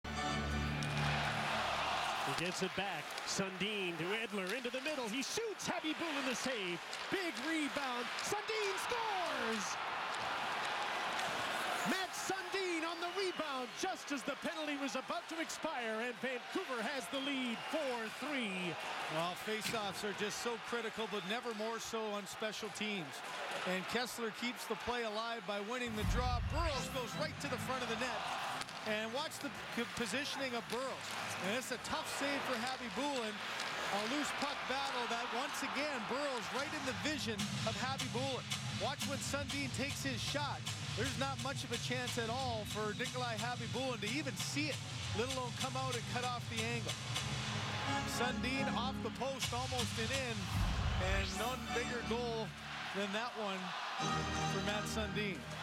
Mats Sundin 4-3 Goal Vs the Blackhawks on Game 6 of the 08 / 09 Playoffs in HD
Mats Sundin rips the shot past Khabibulin to give the Vancouver Canucks a 4-3 lead in game 6. GO CANUCKS GO

Canucks Chatroom Instructions:

Follow this PDF and you'll make it there in under 3 minutes.